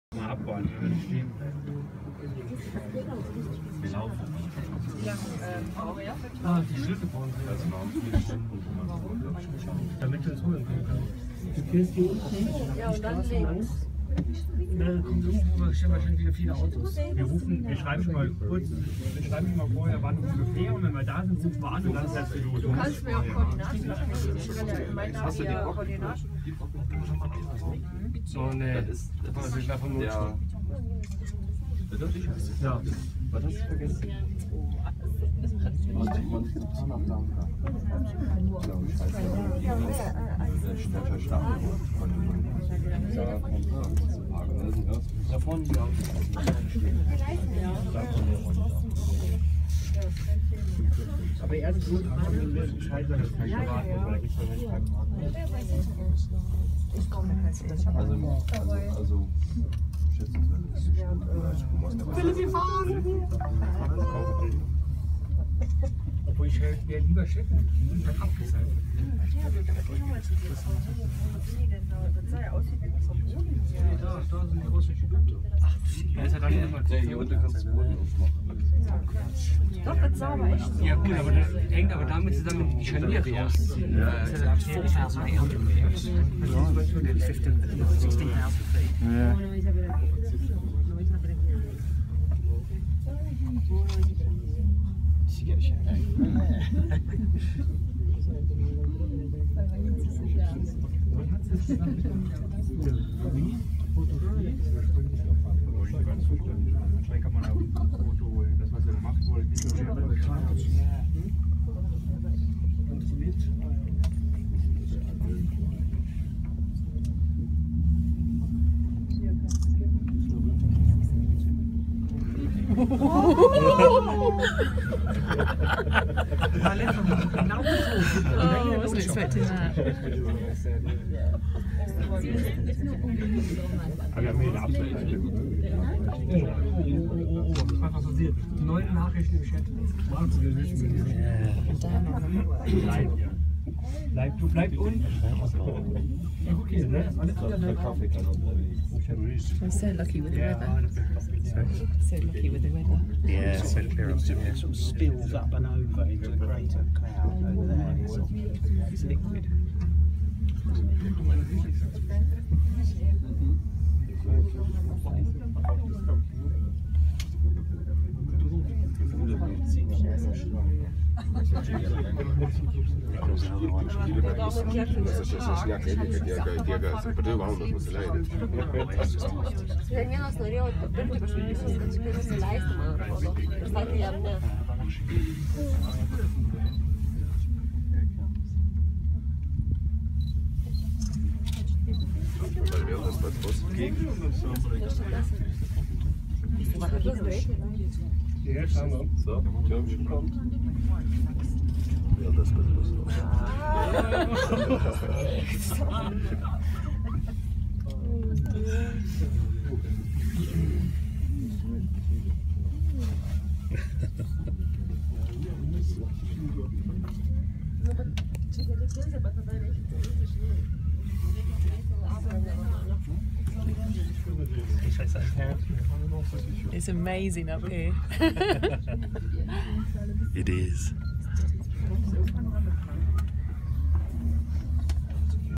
Mount Teide Telerifico
Taking the cable car up to the summit of Mt. Teide, Tenerife. March 2017